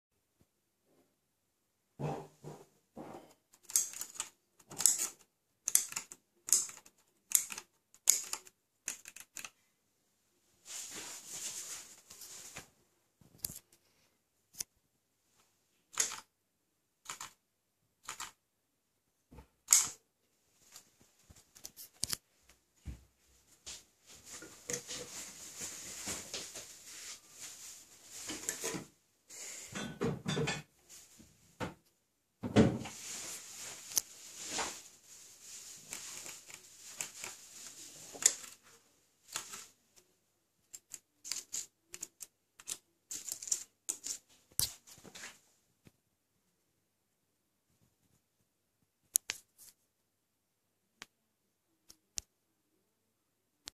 Cue Light System Tests
Cue Light System nearing completion (6 Channels working)

A Level Extended project: A Cue light system, designed for use in the school theatre. The circuit which controls a single channel in the system. Uses a 4017B Decade counter and ULN2803. The fist switch activated the standby light, the second (representing the user) acknowledges the light turning the light to amber, then the controller can give the GO signal (Green). The relay disconnects the user switch when it is not their turn, to stop them interfering with the cues.